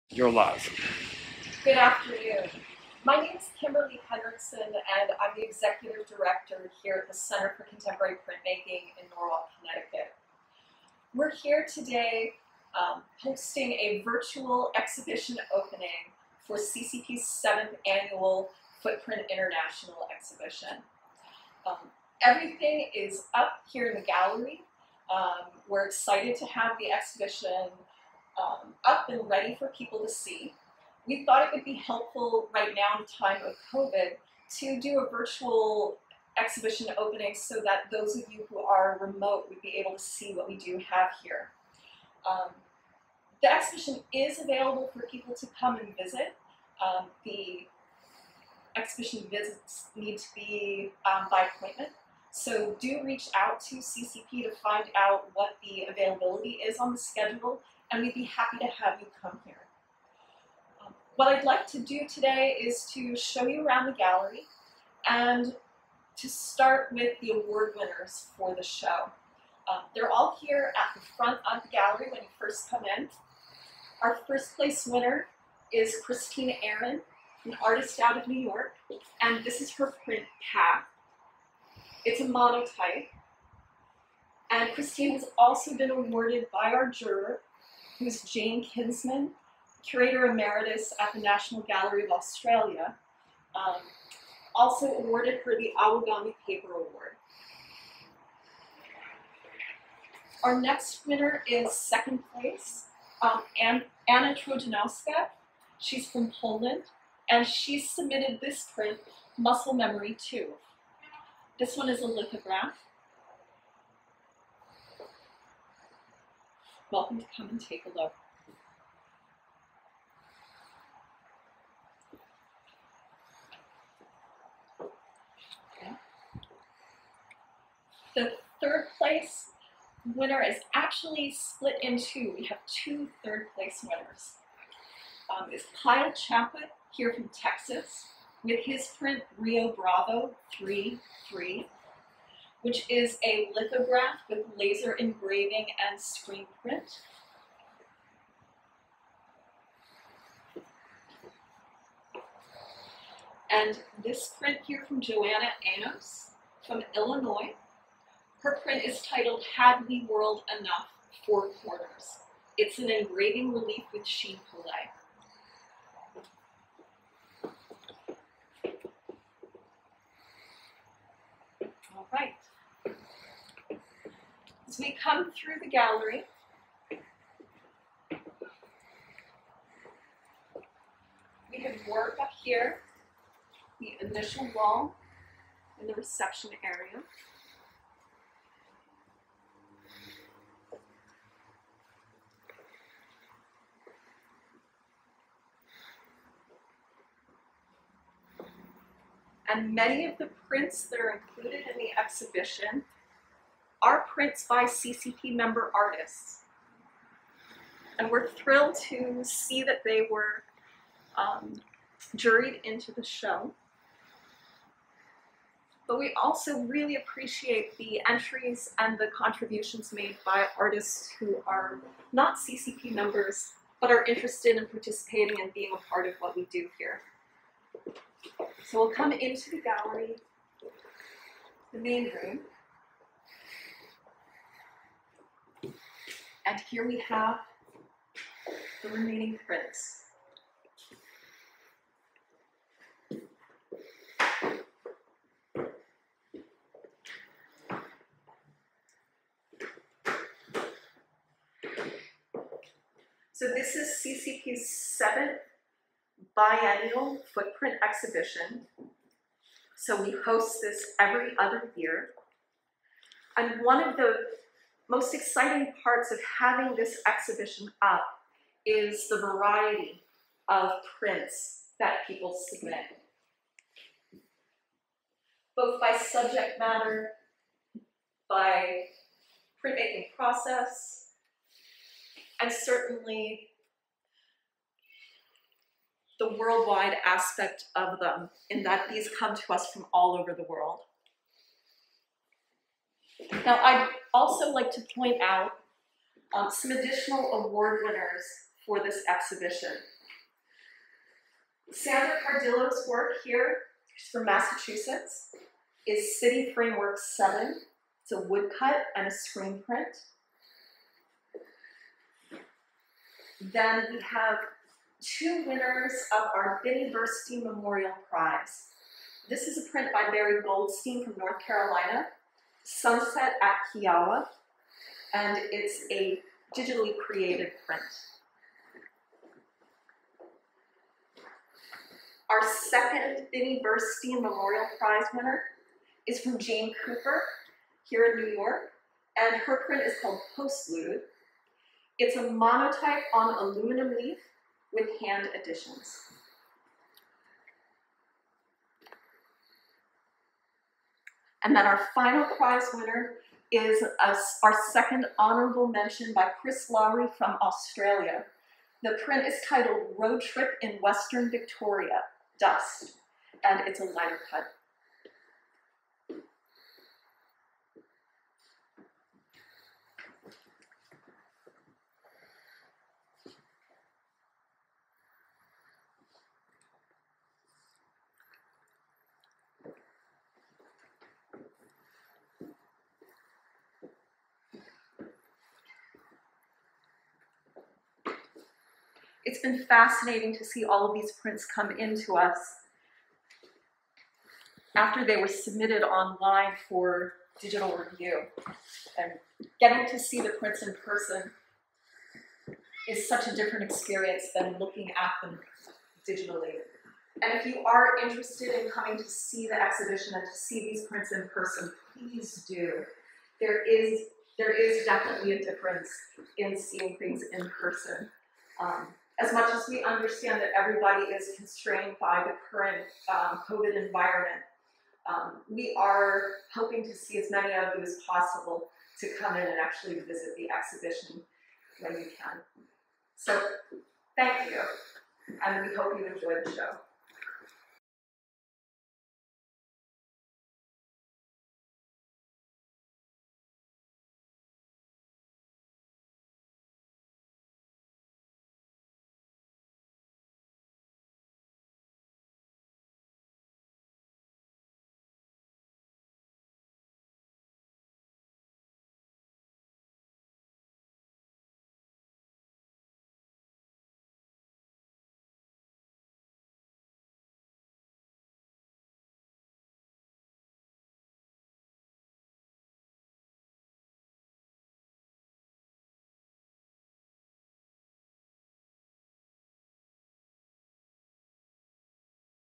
FOOTPRINT2020: Gallery Walk Through
The Center for Contemporary Printmaking’s 7th Biennial FOOTPRINT International Exhibition is open and available for visitors by appointment.

Our Footprint International Competition 2020, is an opportunity for the presentation of current trends and innovations in contemporary printmaking within the bounds of the one square foot (12” x 12”) configuration. The square format was uncommon in art until the advent of abstraction and presents distinct challenges of composition and expression. Within these precise limits, artists must confront a space and create visual order and formal structure.